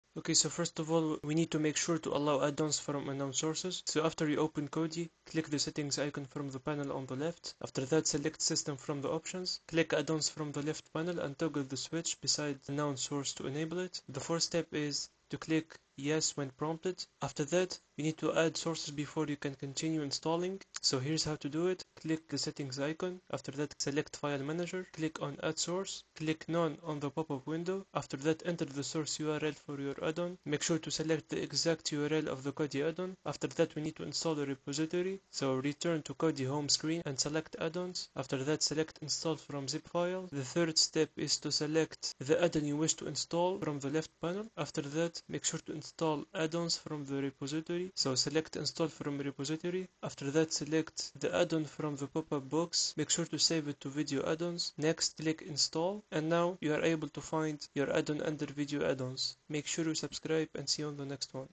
How to Install Addons on Kodi (2025 Beginner’s Guide – Step-by-Step)
Want to unlock the full power of Kodi? In this 2025 tutorial, you’ll learn how to install addons on Kodi easily — whether from the official repository or a custom source.

✅ What You’ll Learn:

How to enable unknown sources in Kodi

Installing addons from the official Kodi repository

Adding third-party sources (like ZIP files or URLs)

Installing video, music, and utility addons

Bonus: Tips to stay safe with third-party addons

📌 Works with Kodi on Windows, Android, Firestick, macOS, and more.